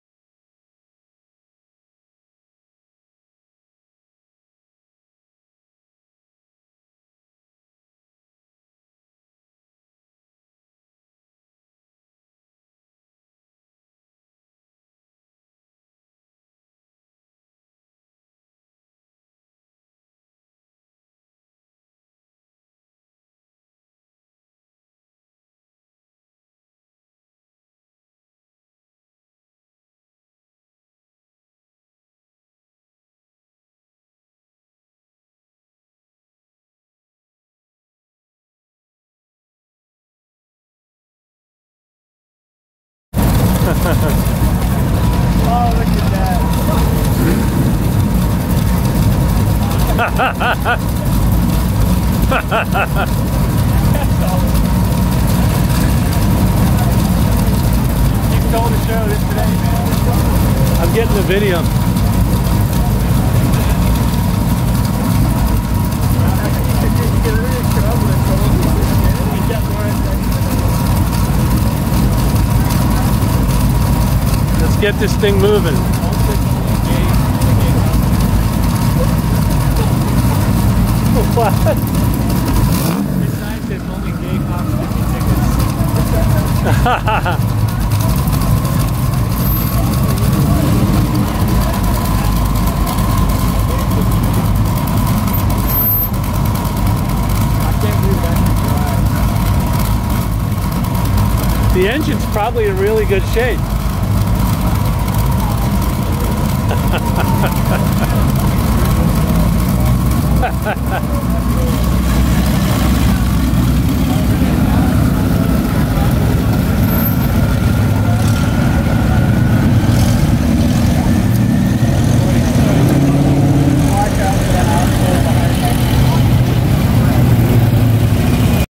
This was the Coolest Car at Cars & Coffee
We loved this VW Bus at Cars & Coffee.  If it were for sale, I would have bought it right away.  Very cool.  There are some photos at the beginning of the video and then a live video.  Very ugly and very creative.